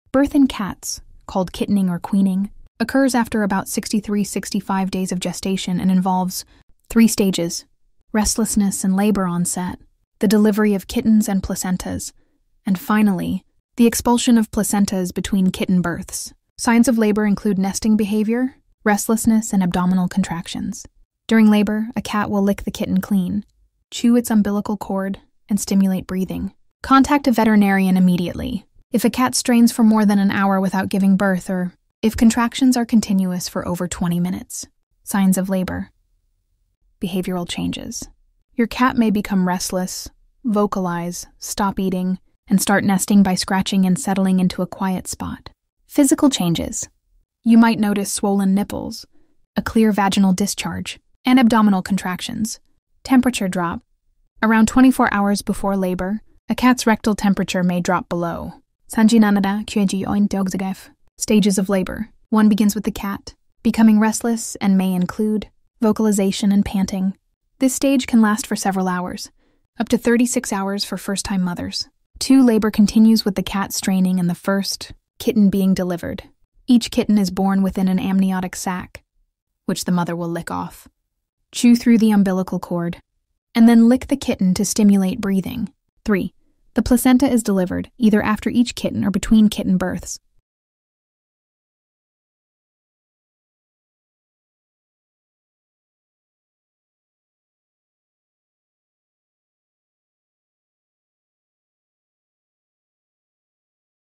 Natural birth in cats.
Birth in cats, called kittening or queening, occurs after about 63–65 days of gestation and involves three stages: restlessness and labor onset, the delivery of kittens and placentas, and finally, the expulsion of placentas between kitten births. Signs of labor include nesting behavior, restlessness, and abdominal contractions. During labor, a cat will lick the kitten clean, chew its umbilical cord, and stimulate breathing. Contact a veterinarian immediately if a cat strains for more than an hour without giving birth or if contractions are continuous for over 20 minutes.
Signs of Labor
Behavioral Changes:
Your cat may become restless, vocalize, stop eating, and start nesting by scratching and settling into a quiet spot.
Physical Changes:
You might notice swollen nipples, a clear vaginal discharge, and abdominal contractions.
Temperature Drop:
Around 24 hours before labor, a cat's rectal temperature may drop below 37°C (98.6°F).
Stages of Labor
1.Begins with the cat becoming restless and may include vocalization and panting. This stage can last for several hours, up to 36 hours for first-time mothers.
2. Labor continues with the cat straining and the first kitten being delivered. Each kitten is born within an amniotic sac, which the mother will lick off, chew through the umbilical cord, and then lick the kitten to stimulate breathing.
3.The placenta is delivered, either after each kitten or between kitten births.